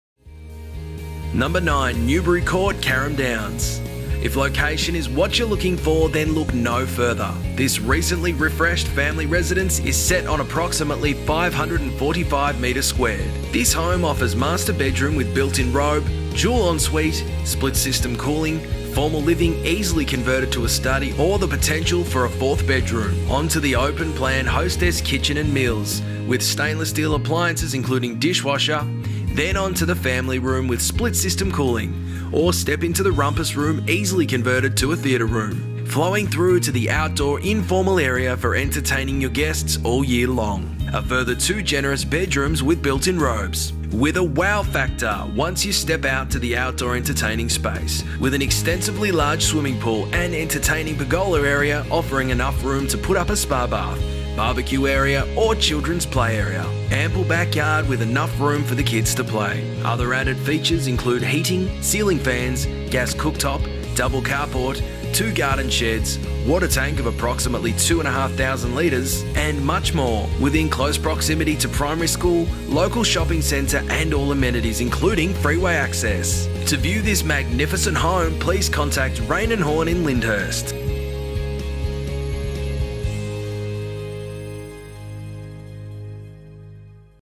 OpenHouseTours video for 9 Newbury Court, Carrum Downs - Presented by Elizabeth Treonze
OpenHouseTours video for 9 Newbury Court, Carrum Downs - Presented by Elizabeth Treonze from Raine & Horne Lyndhurst - The leaders of Real Estate in Lyndhurst